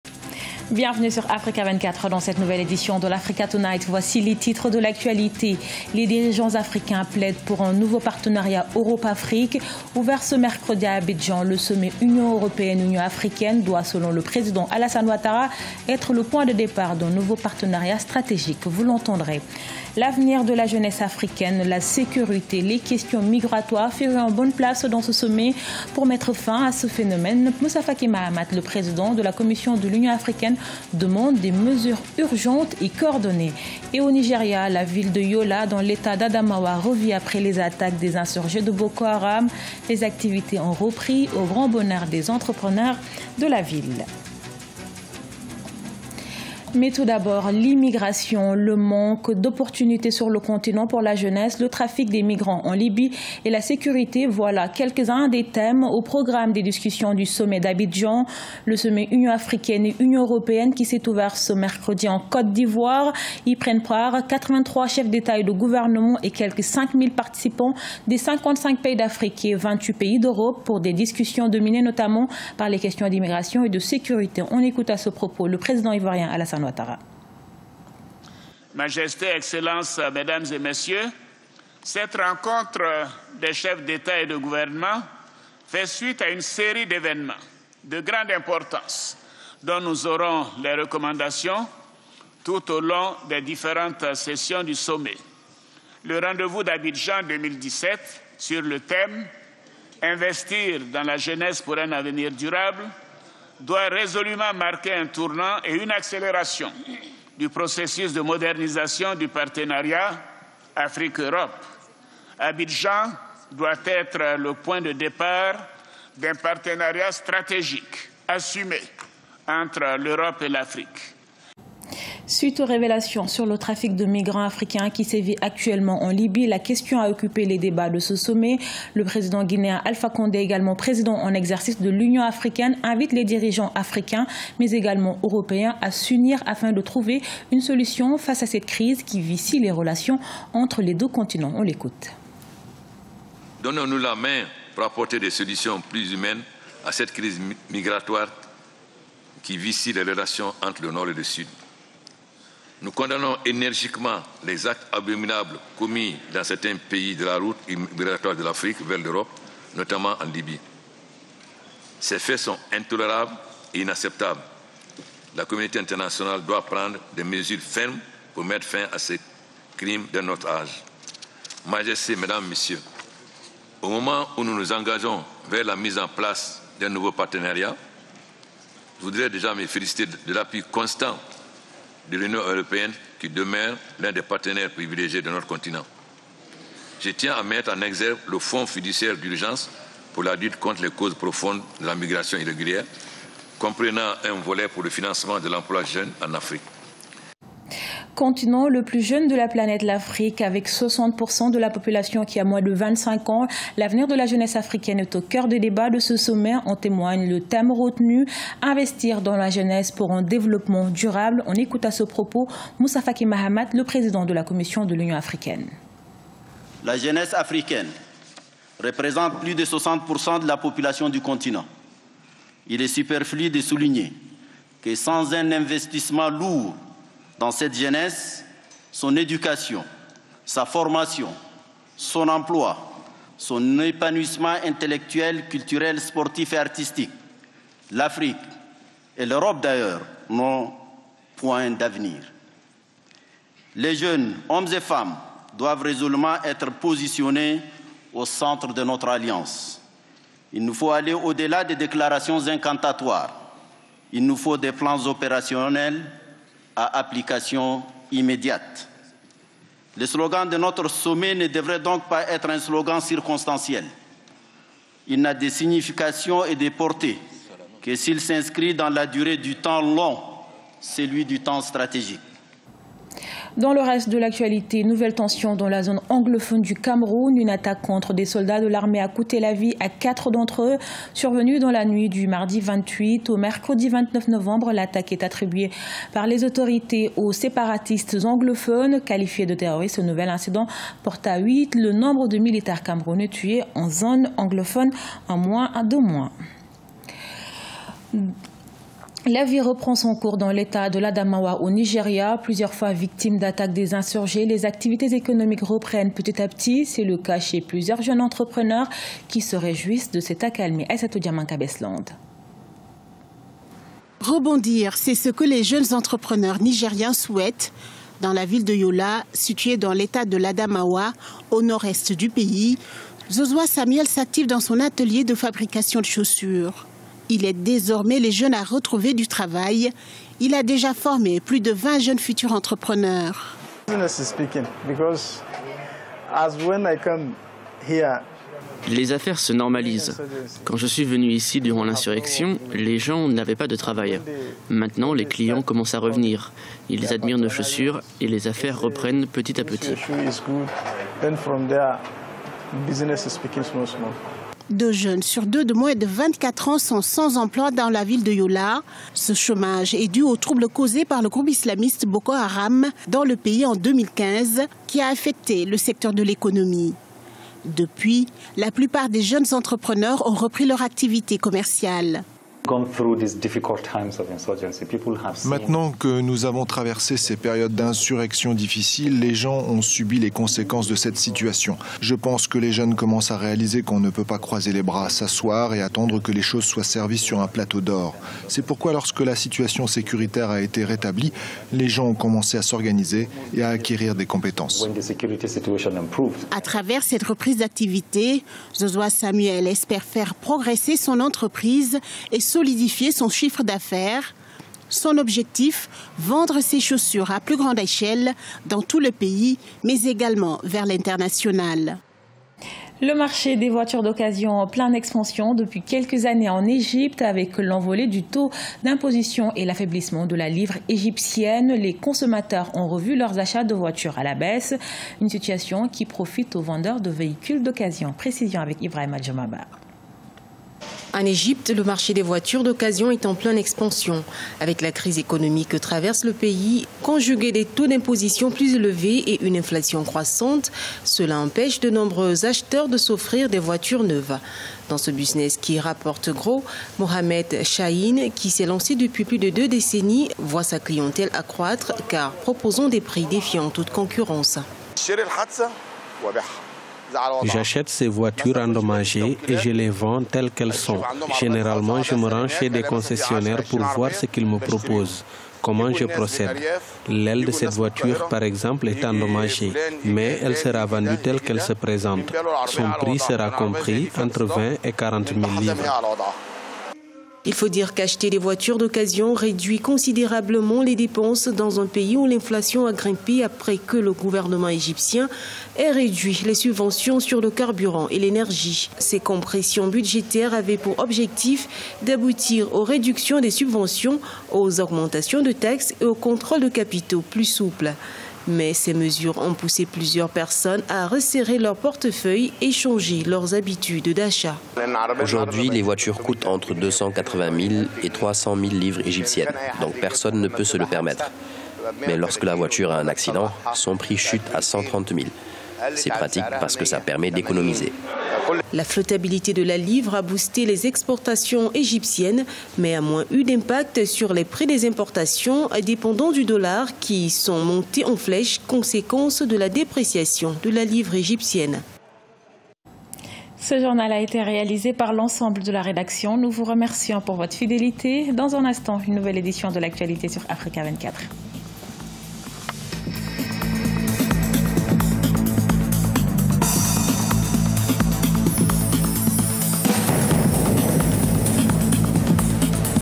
Le Journal du mercredi 29 novembre - 19h GMT
Afrique, ENJEUX DU SOMMET UA-UE À ABIDJAN. Afrique, LES MIGRANTS AU COEUR DU SOMMET UA-UE. Afrique, L'ENGAGEMENT DE L'UA EN FAVEUR DES JEUNES. Nigéria, RELEVER LE NORD GRÂCE À L'ENTREPRENEURIAT.